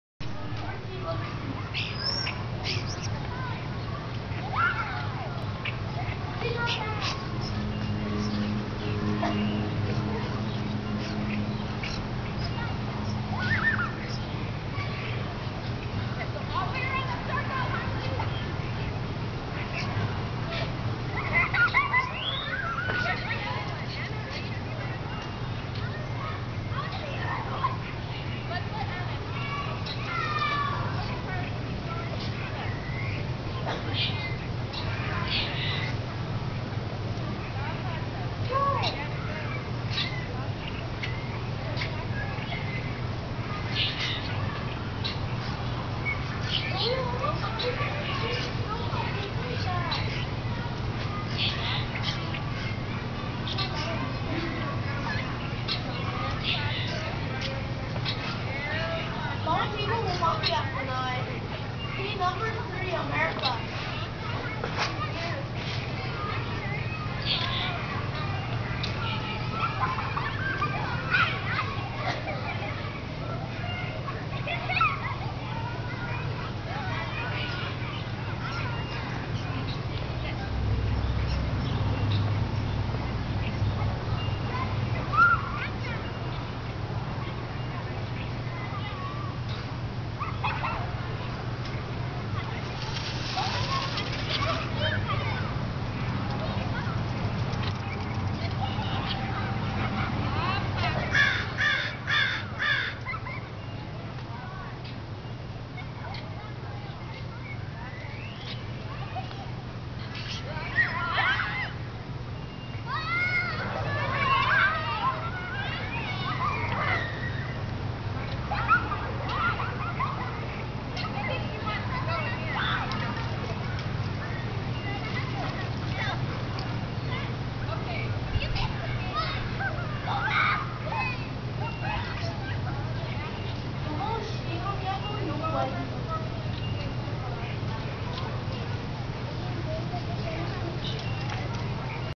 学校に一杯いる鳥の名前は？
この鳥の名前は何だろう。。
冬の間は羽が真っ黒だったのに、最近小春日和が続いたせいか赤い所が出現！
発情期なのかな？